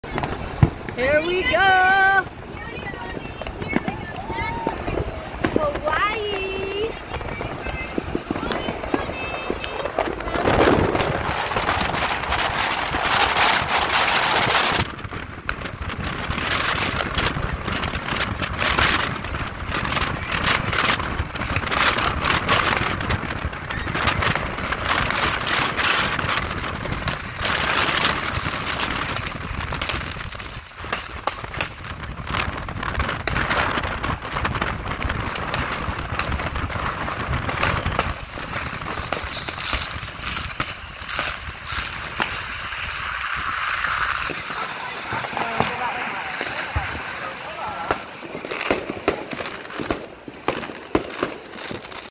Fireworks in Honolulu - New Year Eve from the H1 Highway
You don't even have to leave your car to see the fireworks in Honolulu.  Fireworks start going around 8 PM, but the big show happens at midnight.  Happy New Year!